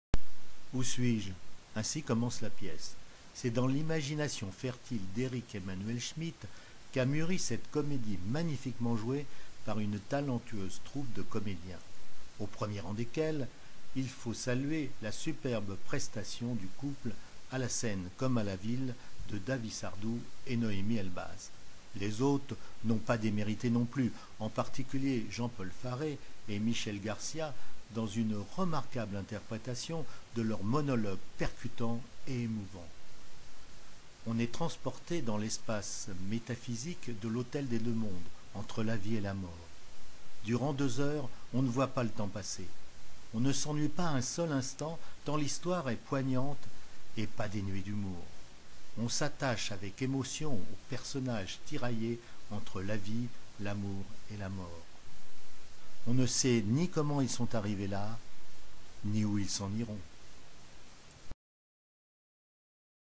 L'hotel des deux mondes Coups d'coeur 045
Ou suis-je ? Ainsi commence la pièce. C’est dans l’imagination fertile d’Eric-Emmanuel Schmitt qu’a muri cette comédie magnifiquement jouée par une talentueuse troupe de comédiens. Au premier rang desquels il faut saluer la superbe prestation du couple, à la scène comme à la ville, de Davy Sardou et Noémie Elbaz. Les autres n’ont pas démérité non plus, en particulier Jean-Paul Farre et Michèle Garcia dans une remarquable interprétation de leurs monologues percutants et émouvants. On est transporté dans l’espace métaphysique de l’hôtel des deux mondes entre la vie et la mort. Durant deux heures on ne voit pas le temps passer, on ne s’ennuie pas un seul instant tant l’histoire est poignante et pas dénuée d’humour. On s’attache avec émotion aux personnages tiraillés entre la vie, l’amour et la mort. On ne sait ni comment ils sont arrivés là, ni où ils s’en iront.